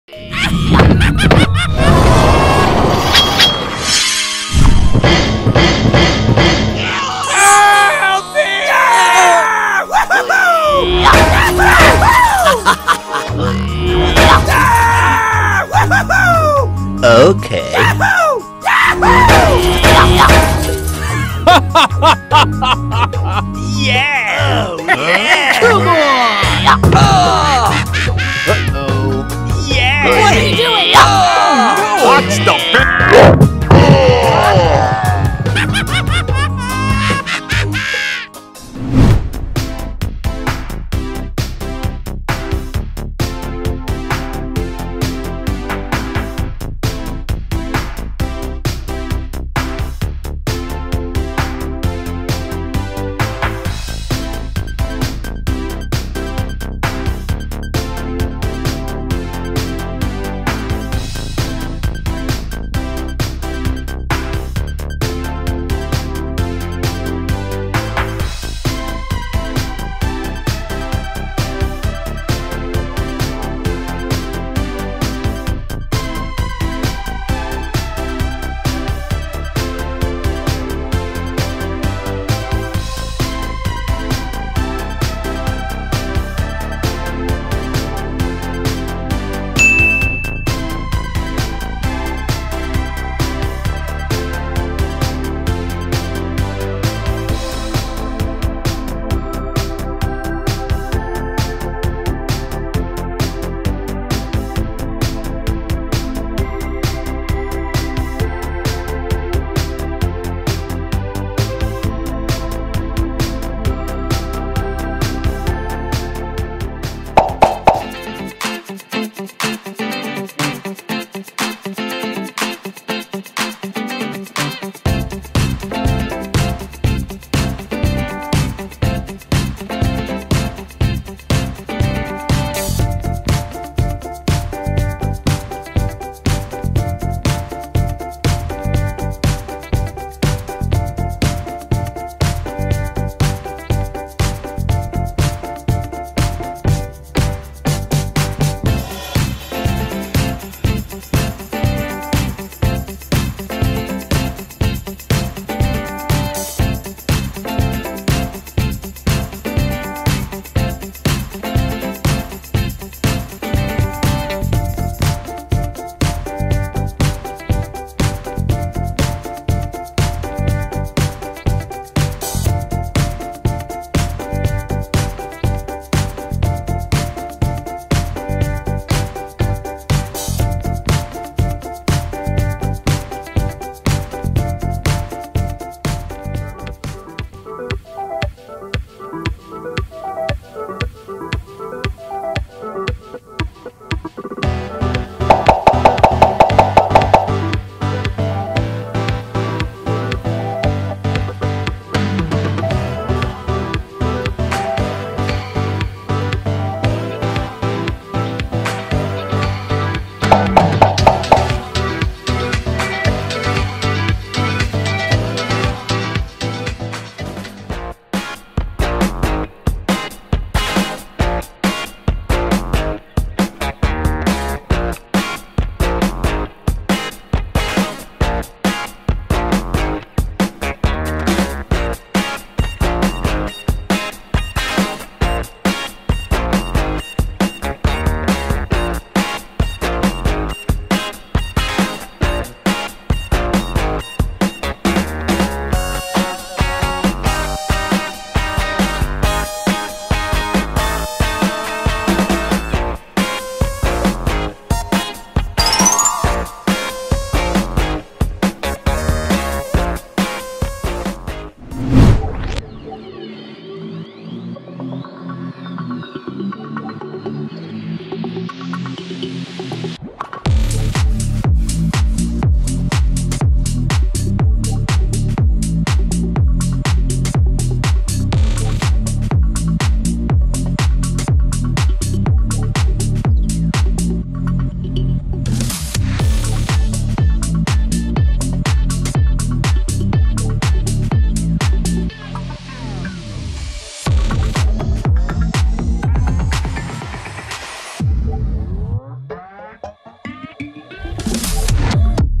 Making Bowling mod Superheroes Deadpool, Wolverine, Venom, Joker with clay 🧟 Polymer Clay Tutorial
00:00 01. Intro - Review Bowling mod Superheroes
00:36 02. Making Bowling mod Superheroes with clay
04:20 03. Review product with clay
This video shows the process of
 from Sky Clay - Satisfying video Timelapse